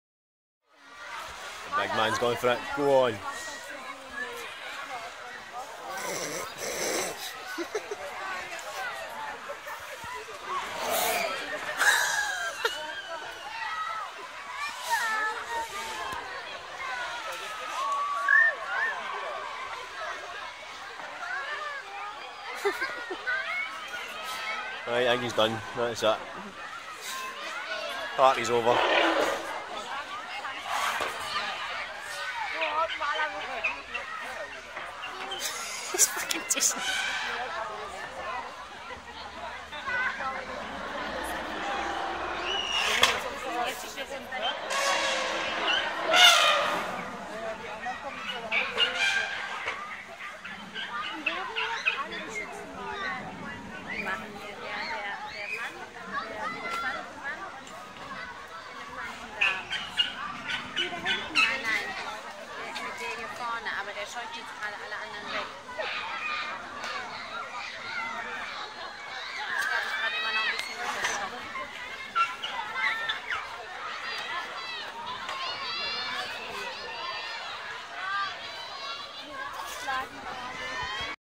Elephant education.
When you go to the zoo and the elephants are real considerate and teach you about the birds and the bees.